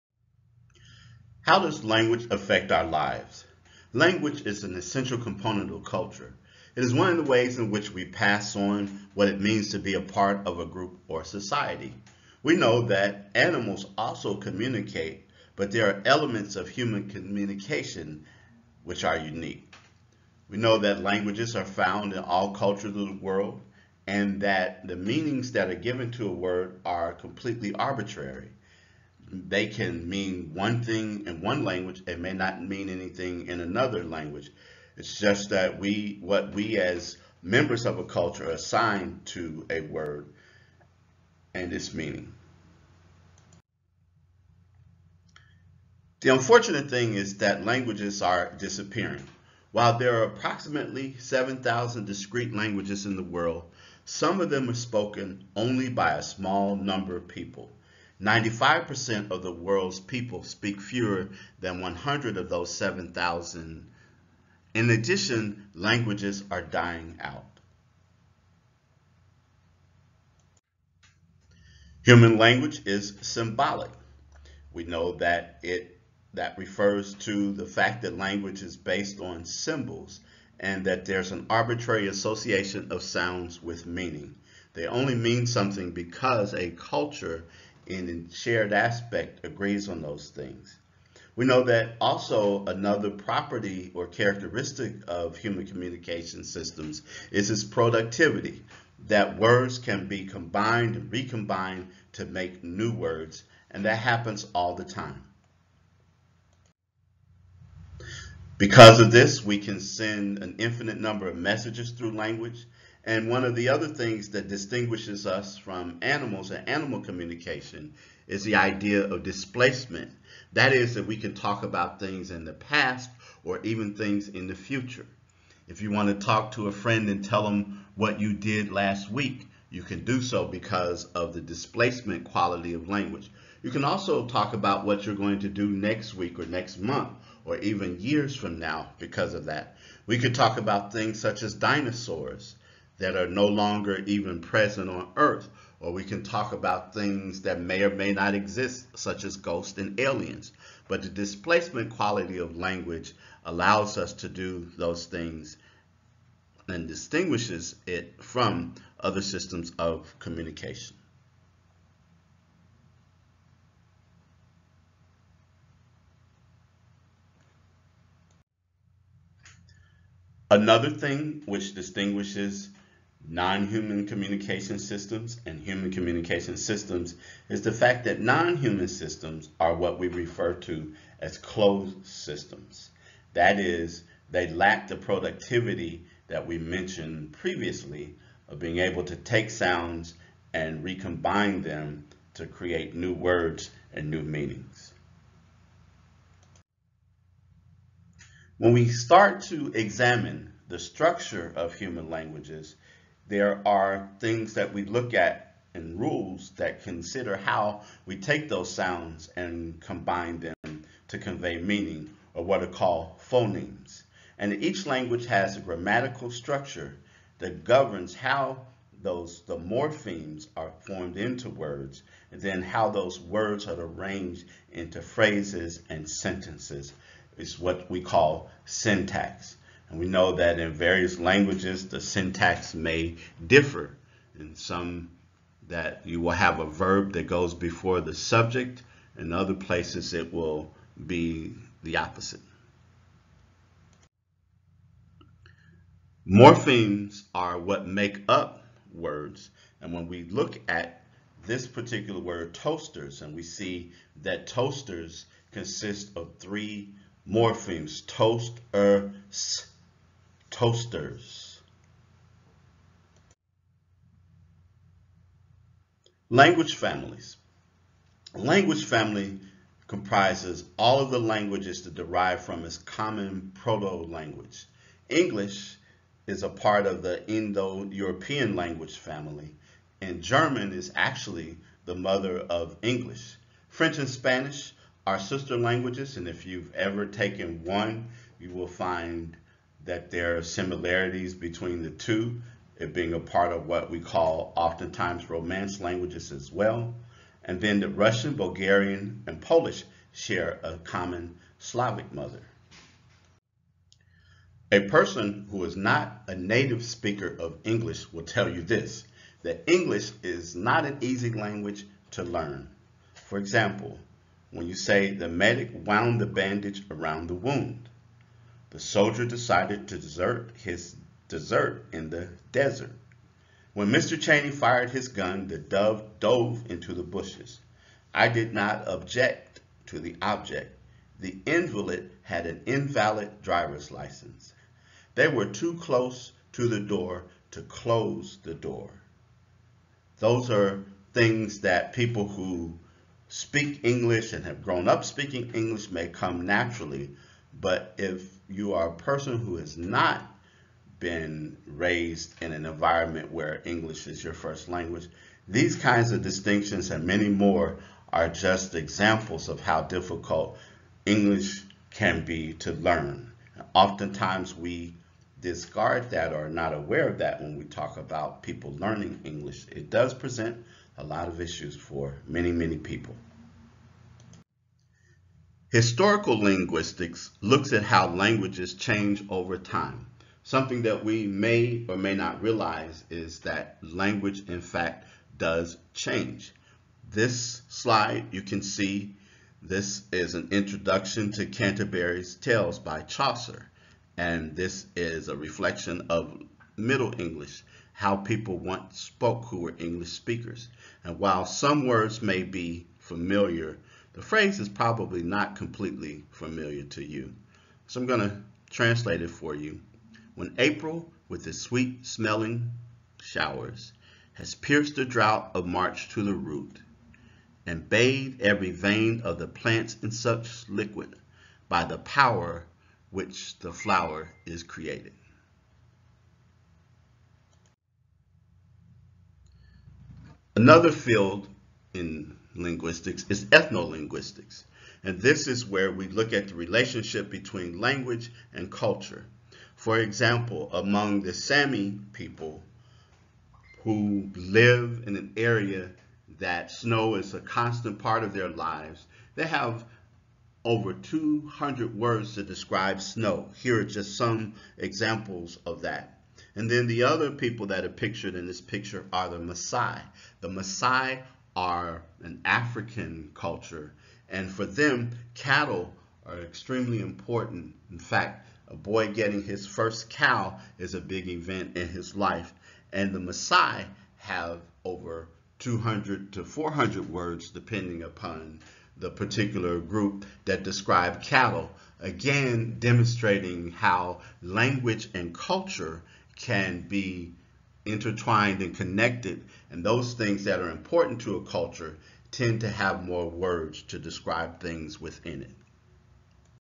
Language lecture one
Language is an essential component of culture. It is one of the ways in which we pass on what it means to be a part of a group or society.